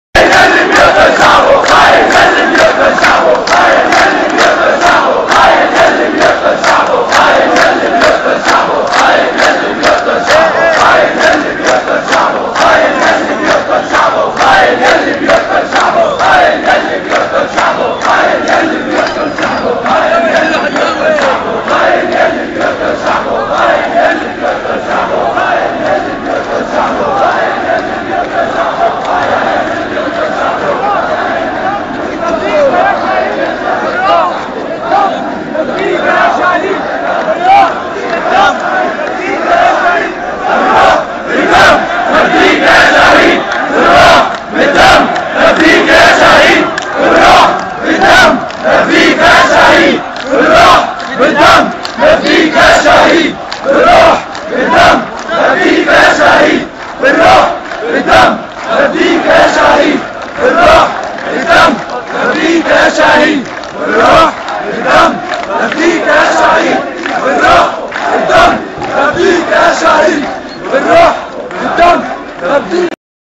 Siria, Damasco, Moadamiyat al Sham, Entierro Multidinario de un manifestante, 05/04/2011
Siria, Damasco, Moadamiyat al Sham, Manifestación multidinaria en el entierro de un manifestante  caido por las balas de miembros del regimen de Bashar al Asad, 05/04/2011